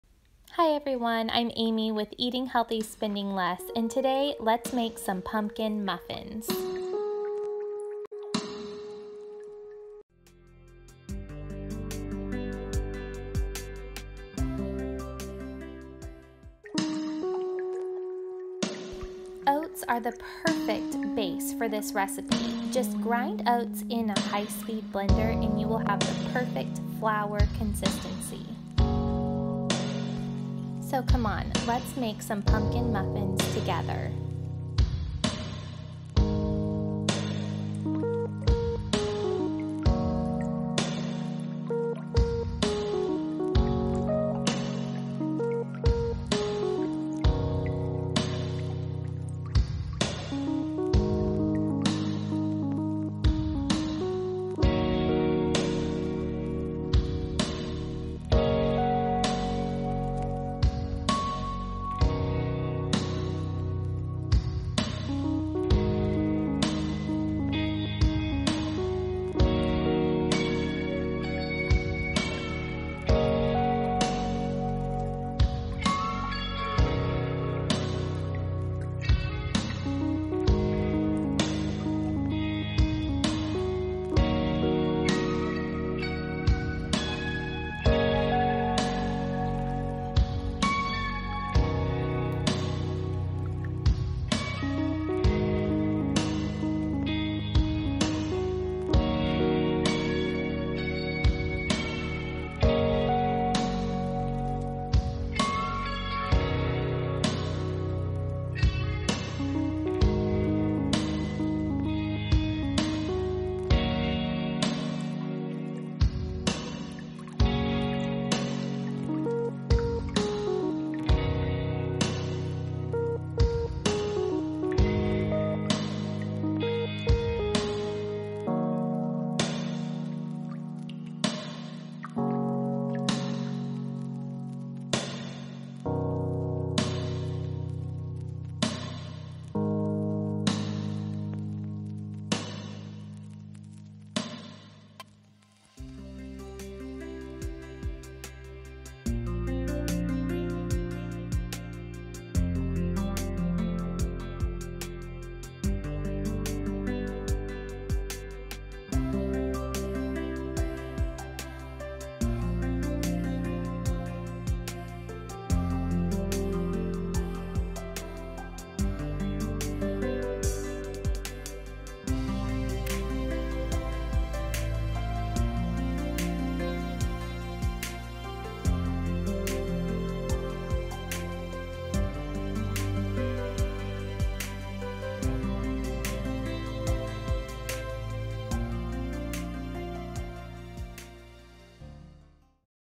Pumpkin Oatmeal Muffins | VEGAN & GLUTEN-FREE | Come bake with me!
Pumpkin oatmeal muffins truly make the fall season come to life!!! It's like fall begins when I can smell these delightful muffins baking in the oven. Don't be fooled, these are just as delicious as any pumpkin muffins recipe.